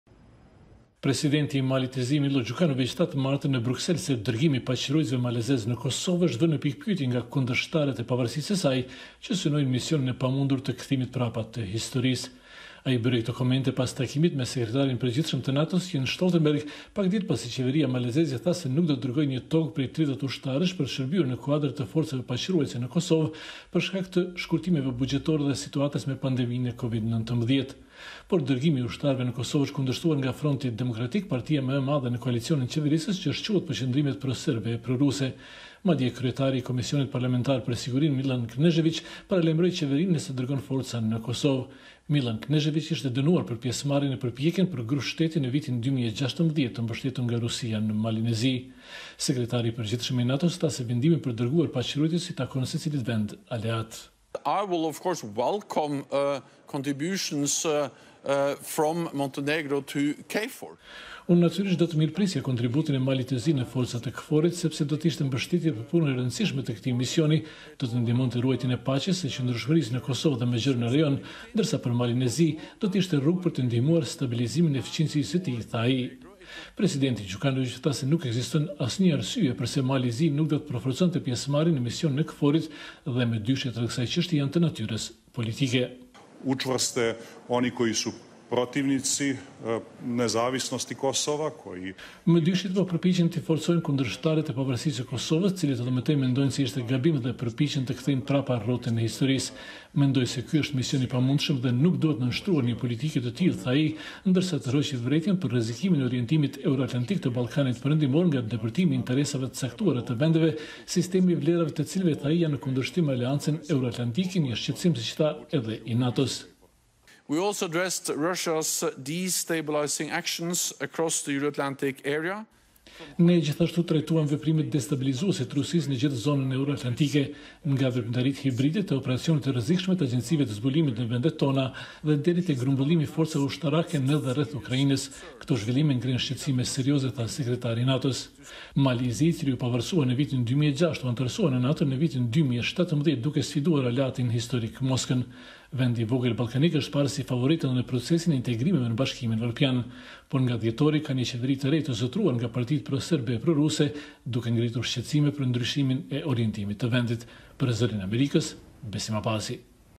Gjukanoviç: Mosdërgimi i paqeruajtësve malazez në Kosovë – i paarsyeshëm
Presidenti i Malit të Zi, Milo Gjukanoviç, tha sot në Bruksel se nuk ka asnjë arsye për të mos dërguar paqeruajtës malazez në misionin e KFOR-it, dhe se mëdyshjet për këtë janë nxitur nga kundërshtarët e pavarësisë së Kosovës. Qeveria në Podgoricë hoqi dorë nga dërgimi i paqeruajtësve në Kosovë nën trysninë e forcave proserbe. Presidenti Gjukanoviç, u takua të martën në Bruksel me Sekretarin e Përgjithshëm të NATO-s Jens Stoltenberg, ku u trajtuan edhe siç u tha, veprimet destabilizuese të Rusisë në të gjithë zonën Euro-Atlantike.